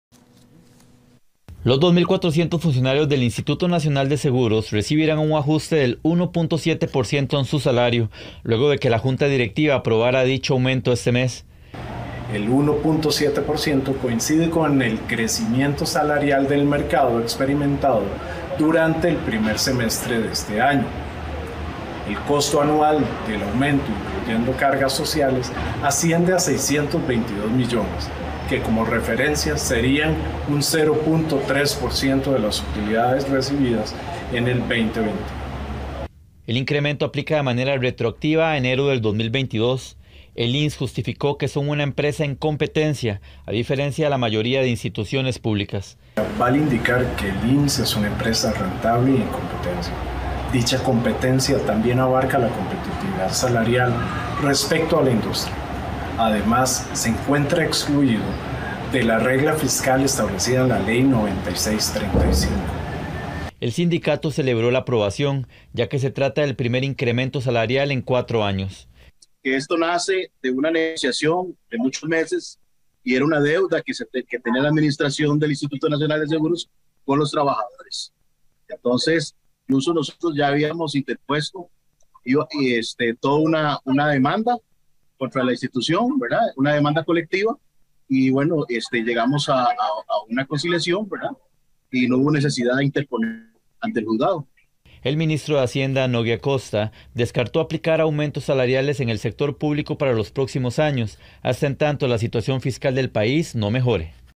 INS aprueba aumento salarial para 2400 funcionarios
Incremento será del 1.7%